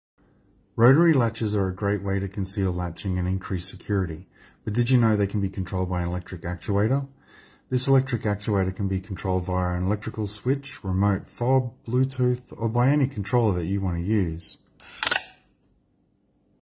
Rotary Latch with Electric Actuator
Rotary Latches are a great way to conceal latching and increase security but did you know they can be controlled by an electric actuator? This electric actuator can be controlled via an electrical switch, remote FOB, Bluetooth or by any controller you want to use.